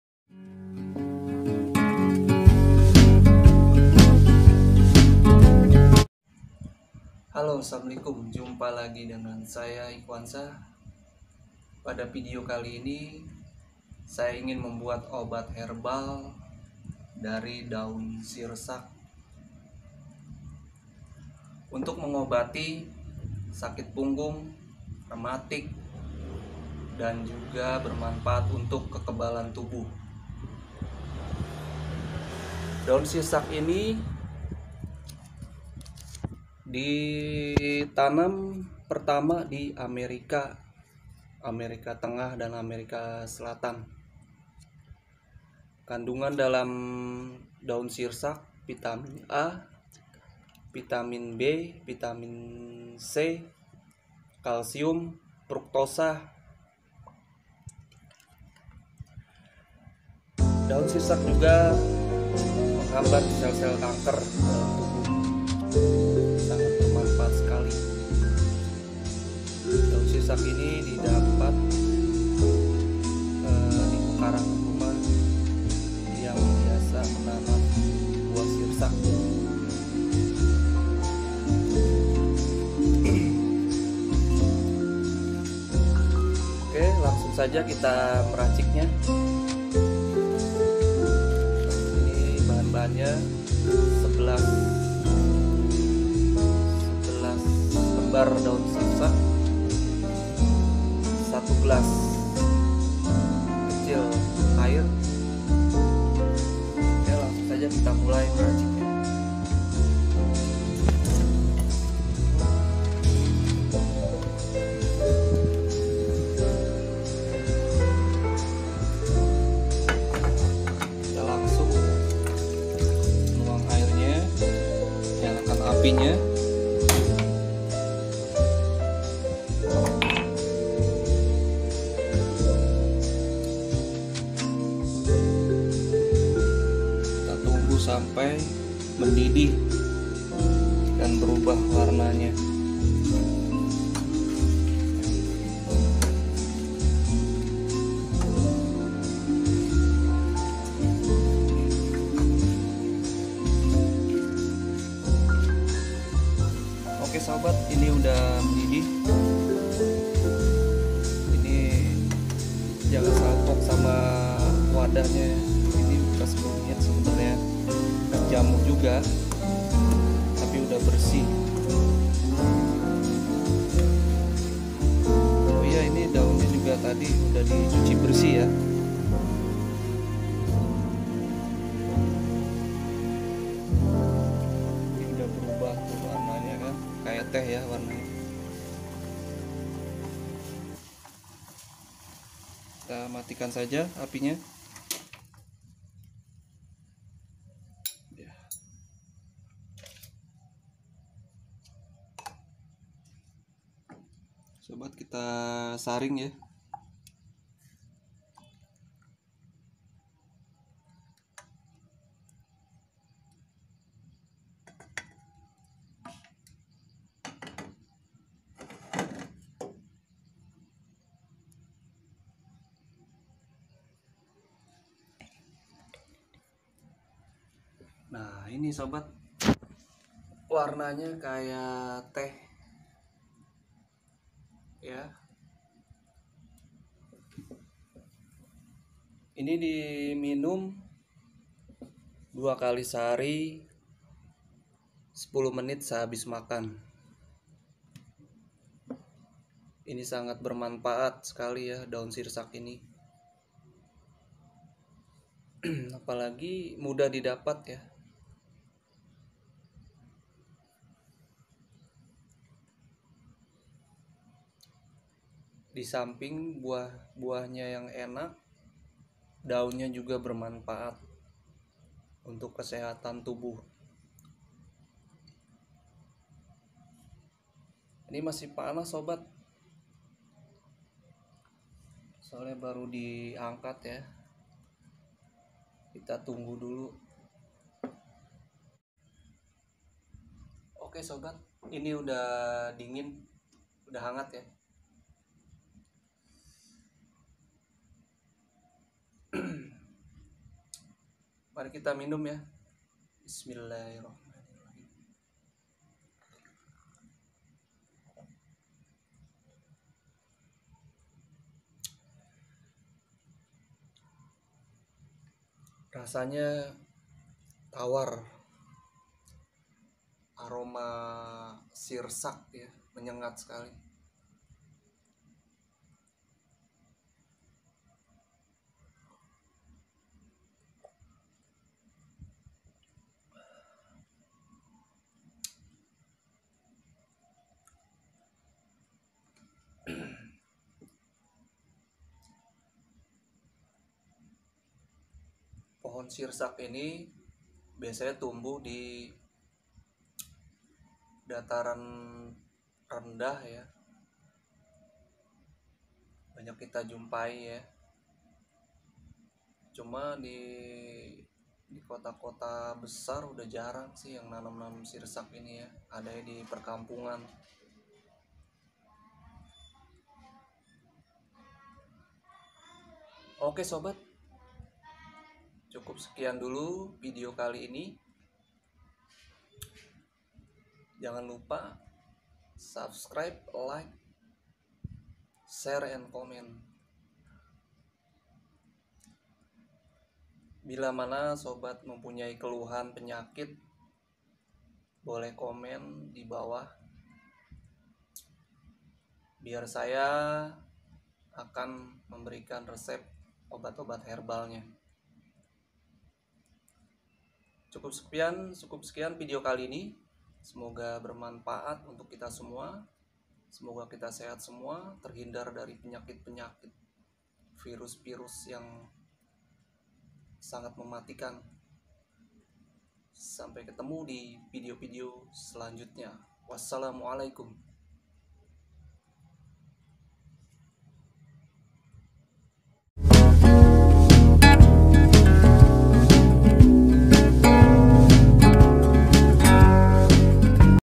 khasiat daun sirsak
obat mujarab dari daun sirsak mengobati sakit punggung, rematik dan kekebalan tubuh caranya ambil 11 daun sirsak cuci bersih lalu rebus dengan segelas air kecil... lebih jelasnya tonton video ini sampai habis